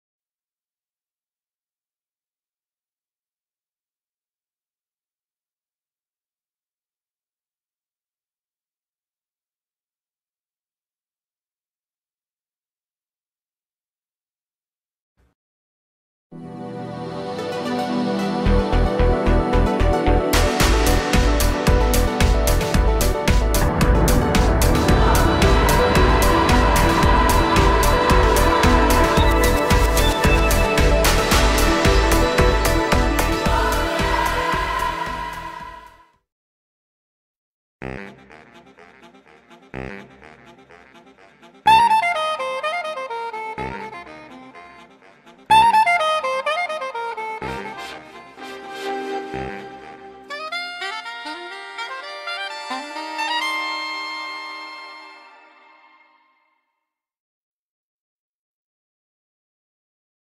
Shochiku logo, Bandal Visual Emotion logo and Madhouse Studios logo
This is logo combo video with both the Shochiku logo, Bandal Visual Emotion logo and Madhouse Studios logo. And you know it would be more interesting? These logos could be use for the upcoming Cardcaptor Sakura: Clear Card movie just before the film starts.